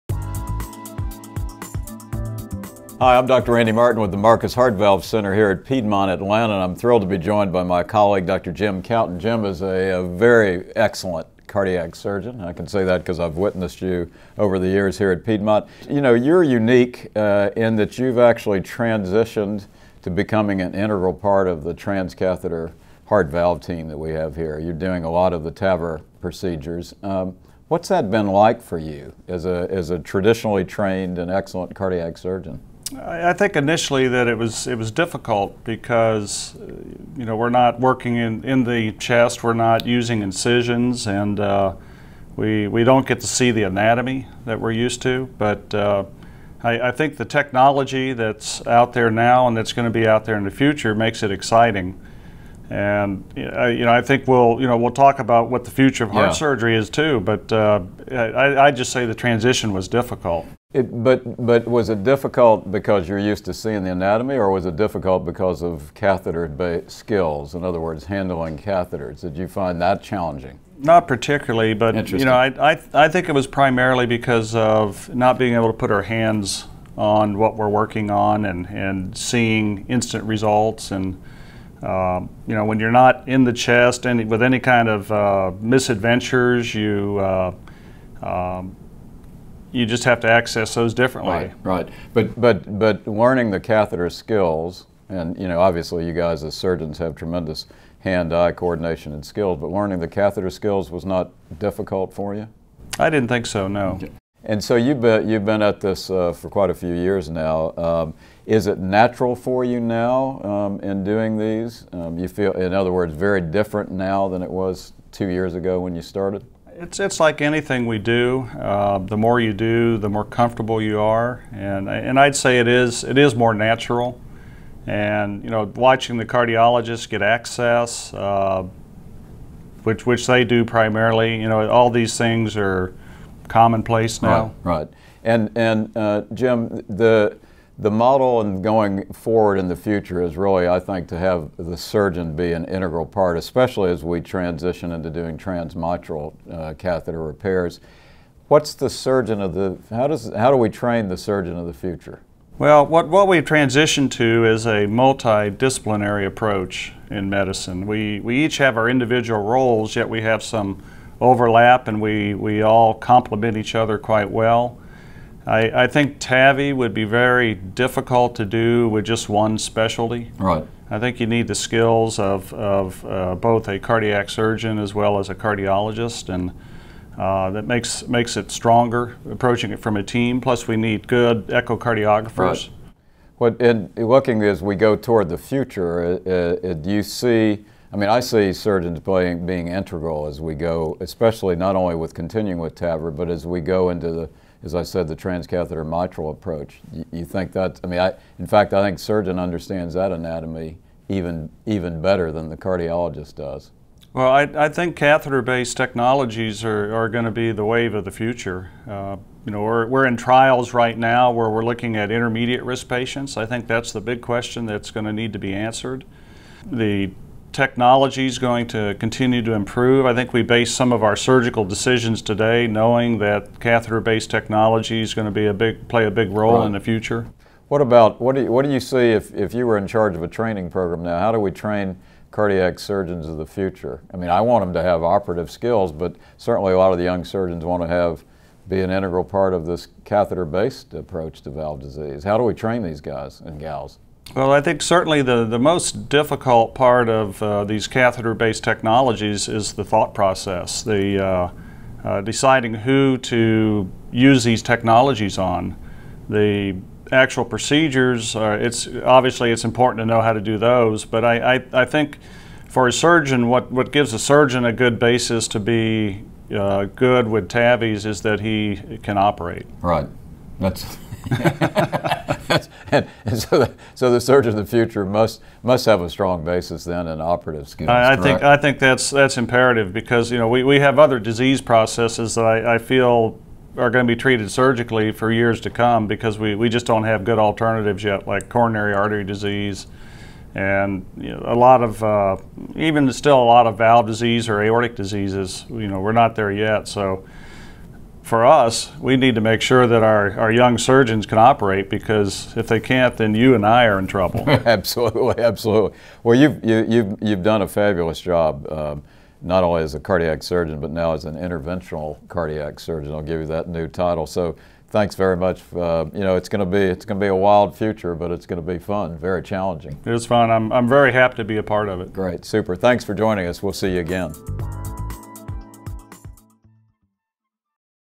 The Evolving Role of a Cardiac Surgeon
How has the role of a cardiac surgeon evolved as a key part of the transcatheter heart team and what is the outlook for the cardiac surgeon of the future? Dr. Jim Kauten, a cardiac surgeon with the Marcus Heart Valve Center, describes his unique experience in a conversation with cardiologist Dr. Randy Martin.